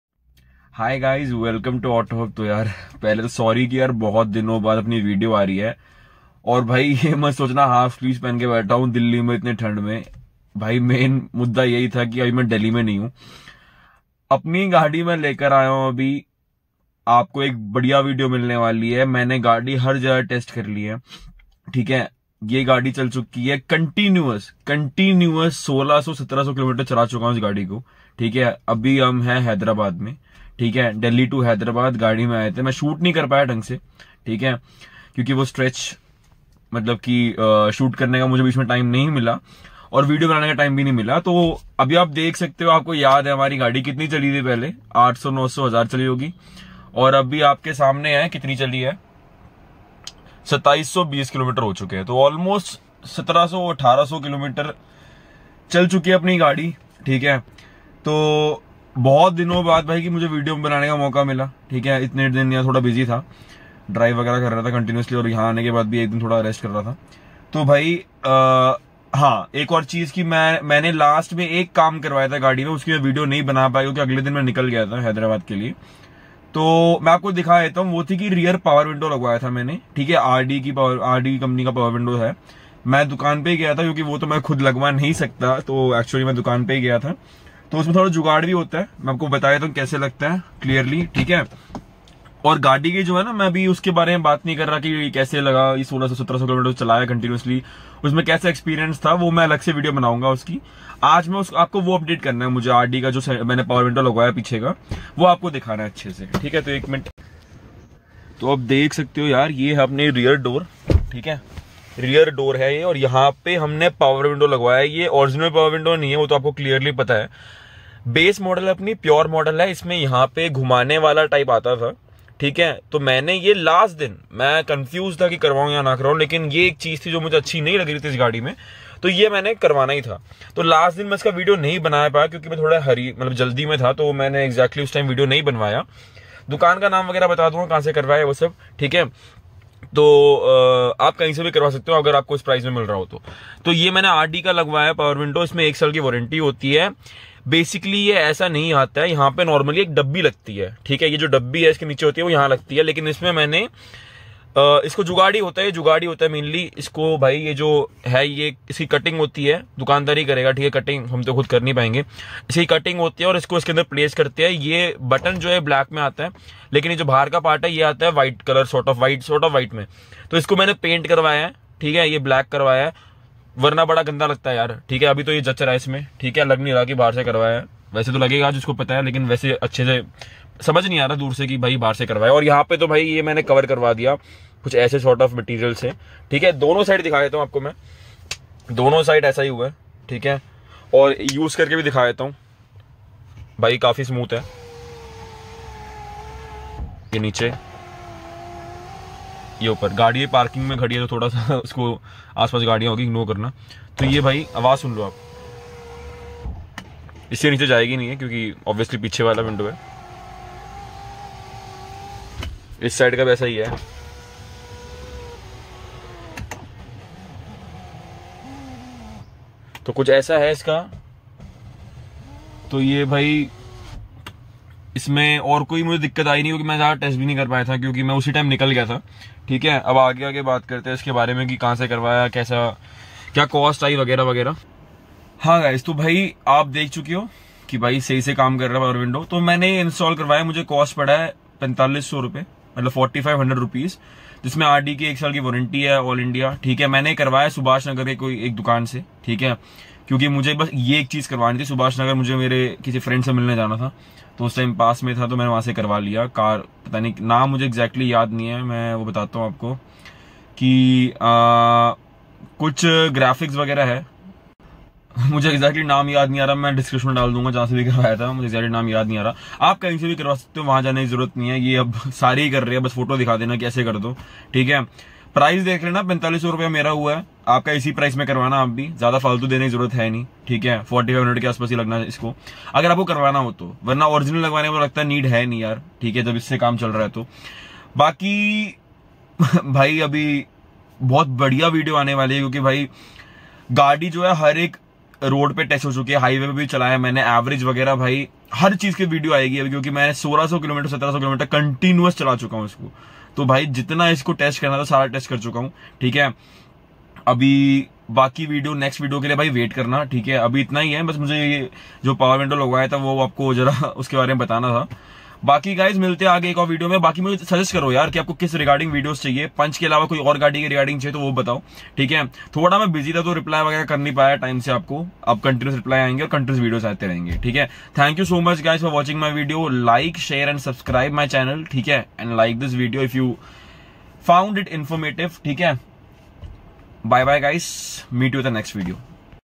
Tata punch pure model modified. Rd power window oem style installed. Tata punch pure model.
Power window complete price- 4500
Shop- advance car
I.had only installed power window from this shop. Other than that nothing was installed from that shop. Neither i recommend any shop.
All of the other items were bought and installed by myself